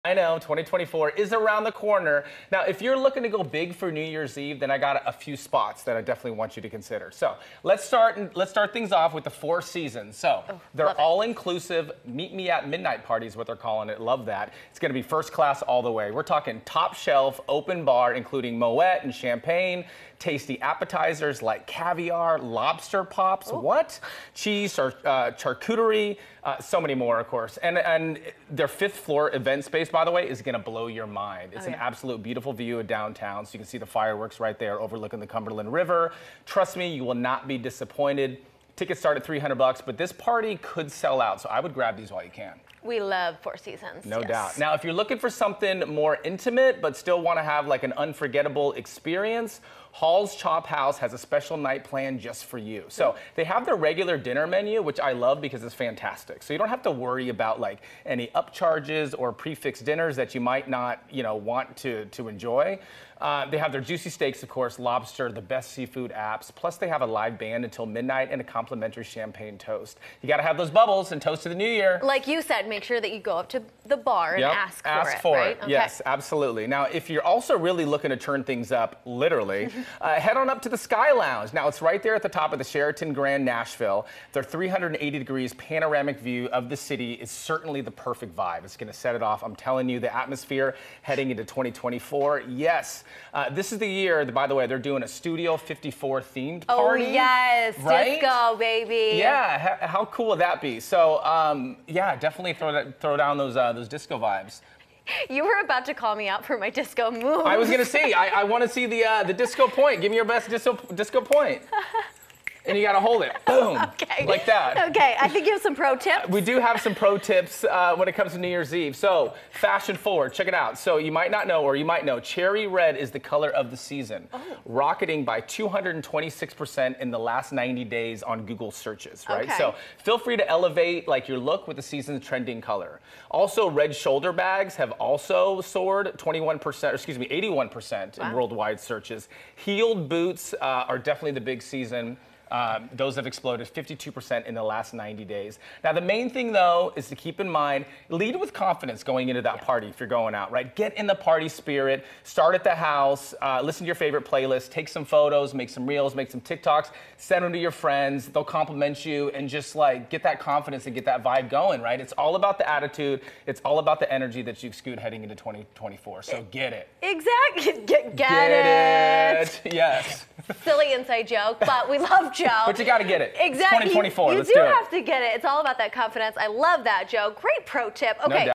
What are your plans for NYE? Here are a few ideas
Joe Breezy has got you covered when it comes to making plans for New Year's Eve, but you have to get your tickets early -- they're going fast!